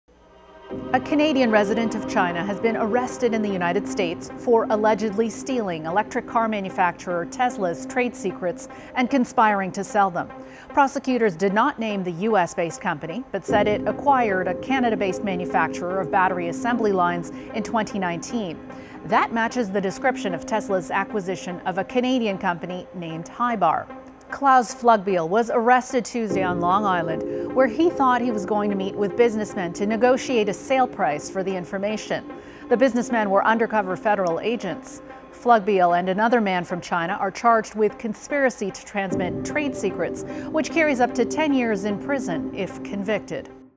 Canadian living in China allegedly stole Tesla trade secrets: US prosecutors
A Canadian resident of China has been arrested in the U.S. for allegedly stealing electric car manufacturer Tesla’s trade secrets and conspiring to sell them to undercover law enforcement officers.